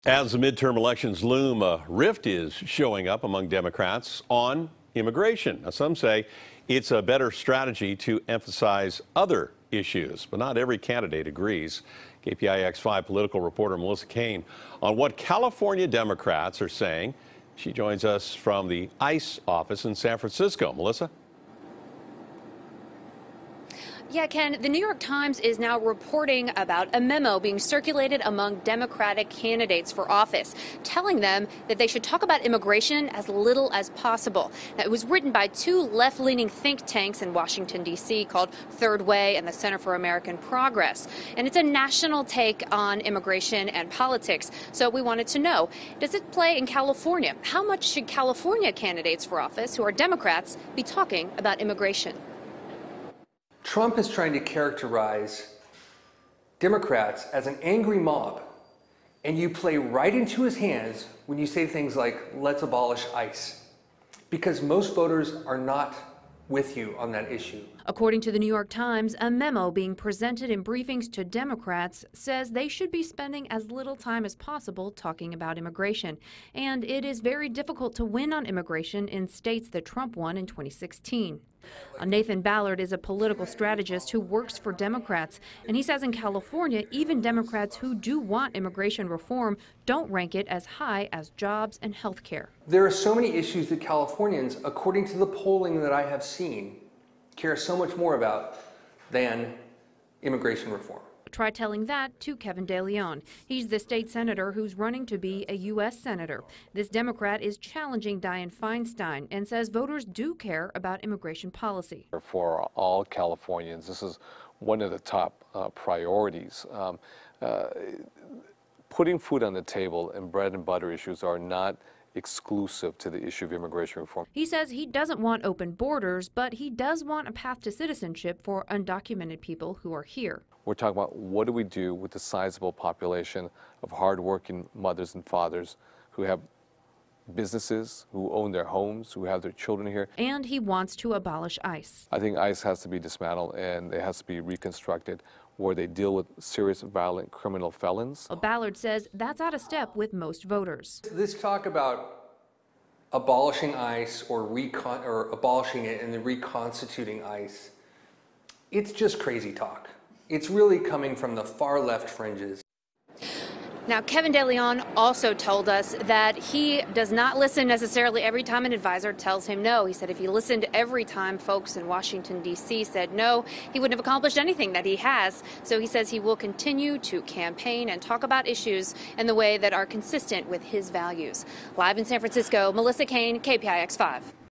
CA Democrat Not Aligned With Memo On Immigration From Campaign Strategists
As the midterm elections loom, a rift is developing among Democrats on immigration. Some are saying it’s better to emphasize other issues, but as Melissa Caen tells us, in California, not every candidate agrees.